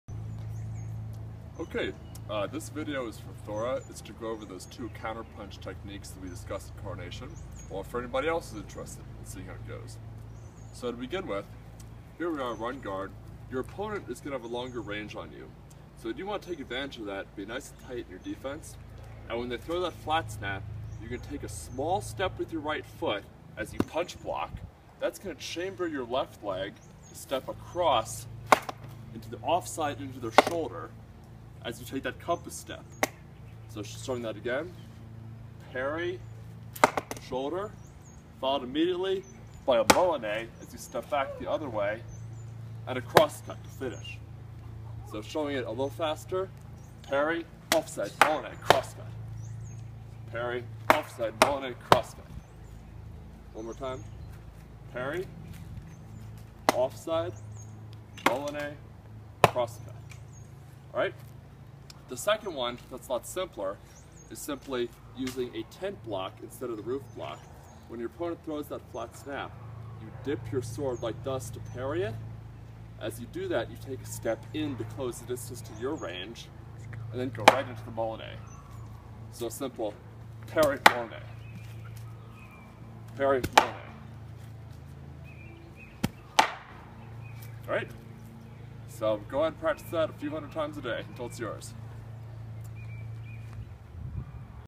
Sword and Shield Technique video: 2 Counter attacks
These are two of my favorite counters to a flat snap, and I taught these at Coronation last weekend. Thora asked for a video so she can review the mechanics, so this is being broken down for a shorter fighter.

If your opponent has a range advantage, it is important that you take a closing step as you parry, taking you into your ideal range to throw the riposte(s). Given my range, I often omit this closing step.